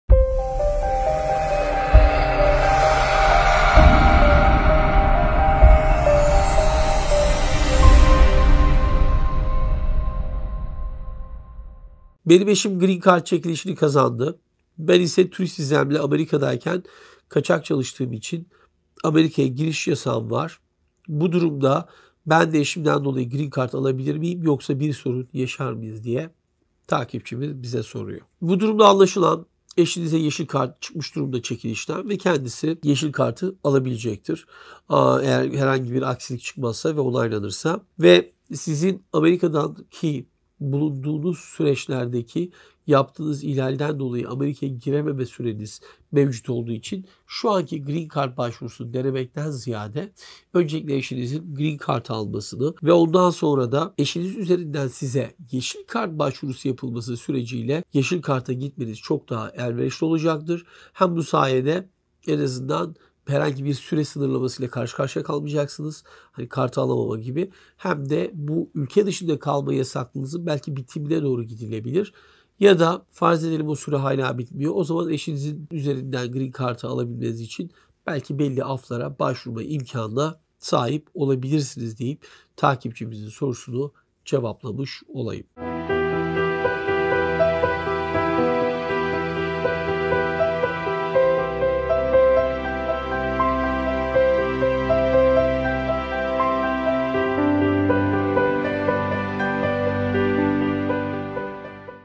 AMERİKA’DA KAÇAK ÇALIŞANLAR GREEN CARD KAZANIRSA KART ALABİLİR Mİ ?
Soru: Benim eşim Green Card çekilişini kazandı. Ben ise turist vizemle Amerika’dayken kaçak çalıştığım için Amerika’ya giriş yasağım var. Bu durumda ben de eşimden dolayı Green Card alabilir miyim yoksa bir sorun yaşar mıyız ?